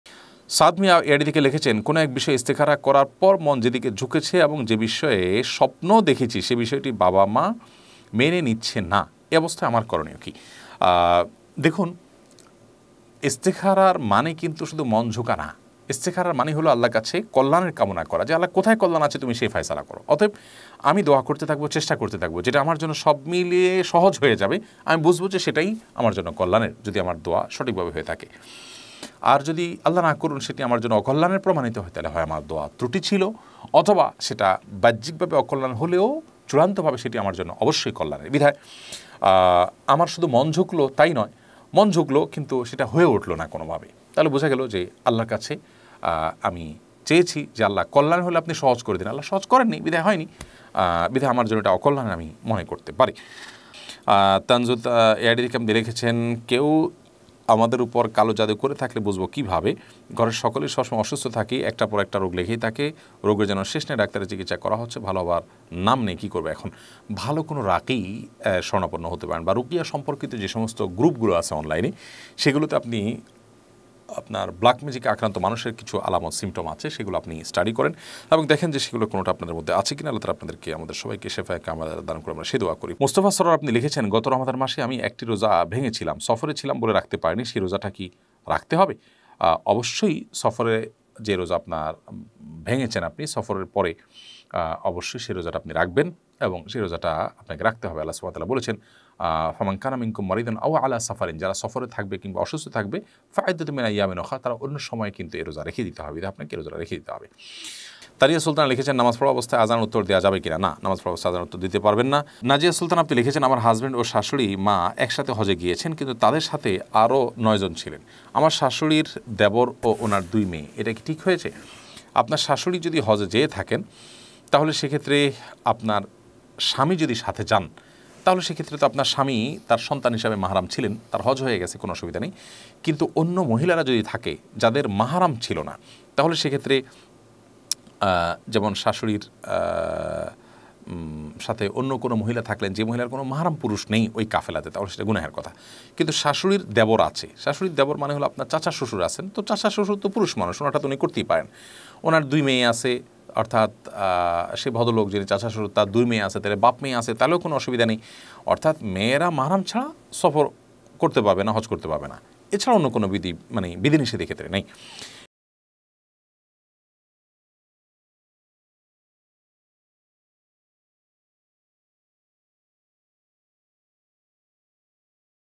ইস্তেখারার পর যেদিকে মন ঝুঁকছে সেটিকে বাবা মা মানছে না , আমার করণীয় কী?
ইস্তেখারার পর যেদিকে মন ঝুঁকছে সেটিকে বাবা মা মানছে না , আমার করণীয় কী? new waz II waj II Sheikh ahmadullah